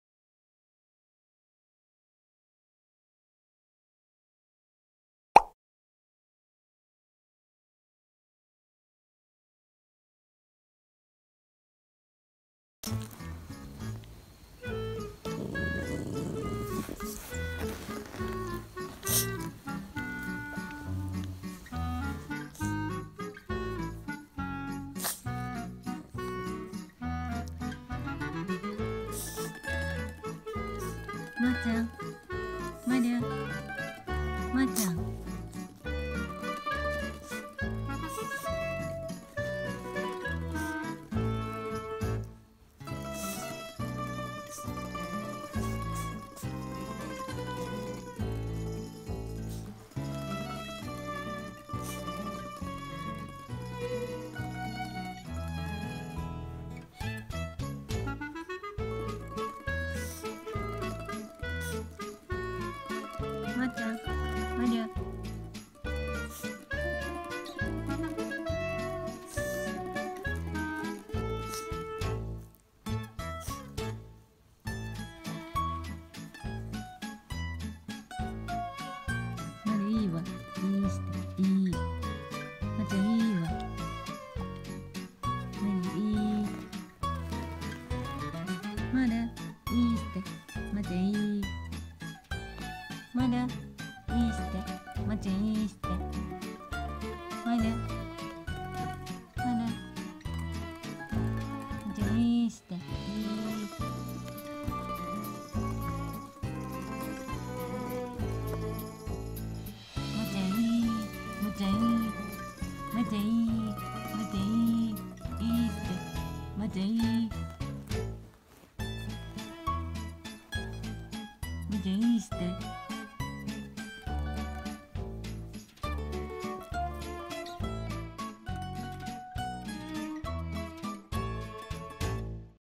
【ヨークシャーテリア】まる 靴下
長男まる　足を舐めるので靴下を履かせられました。